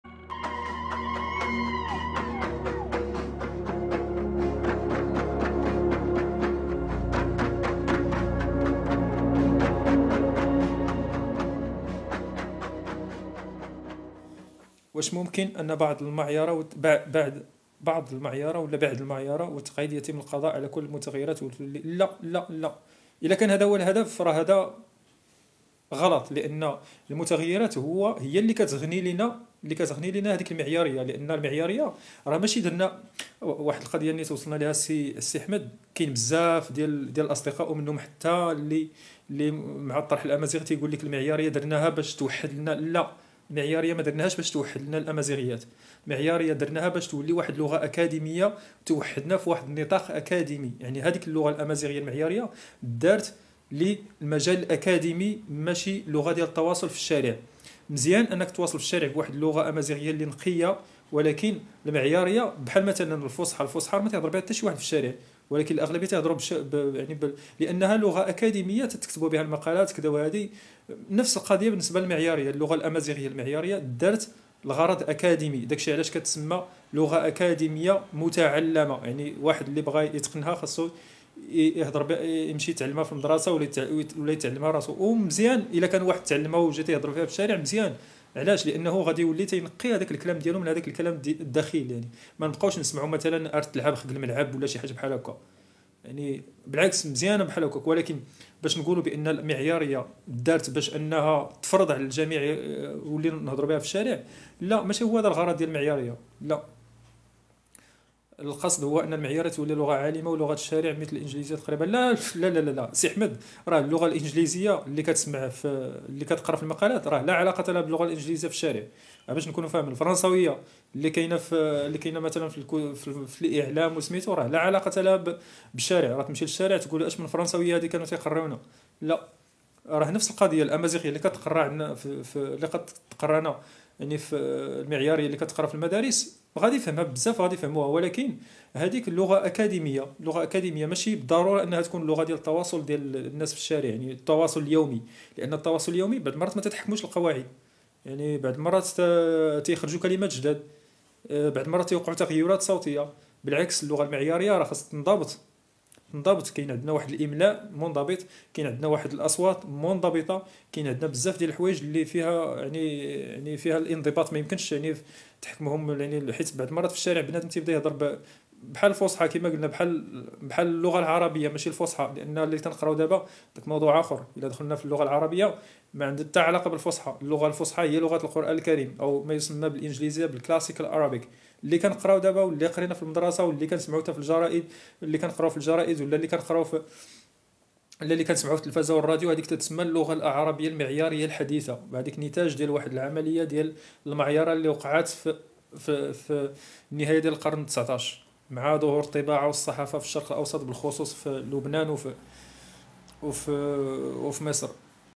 واش اللغة الأمازيغية المعيارية توحد اللهجات الأمازيغية ؟؟
دردشة حول اللغة الأمازيغية المعيارية وعلاقتها  باللهجات الأمازيغية.

ماتنساوش الاشتراك فالقناة وتسجيل الإعجاب ودعم المحتوى.